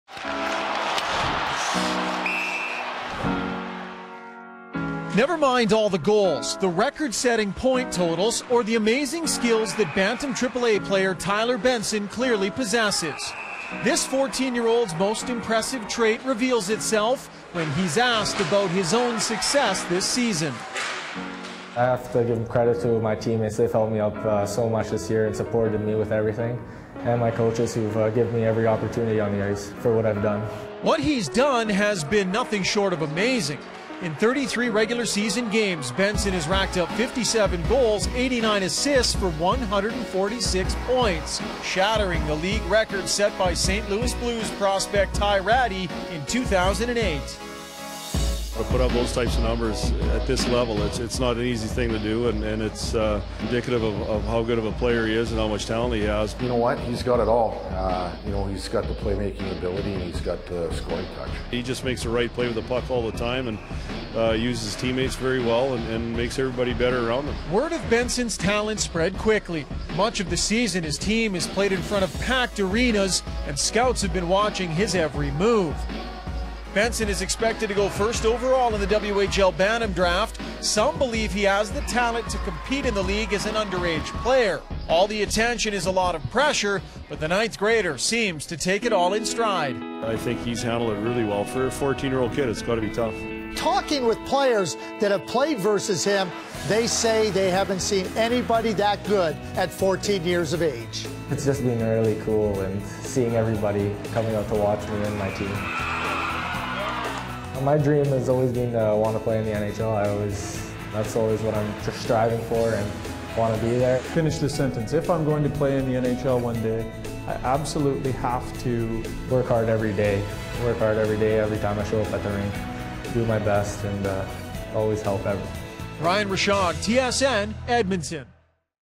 Tyler Benson - TSN WHL Draft Feature
Throwback TSN video of Tyler Benson leading up to his WHL Draft.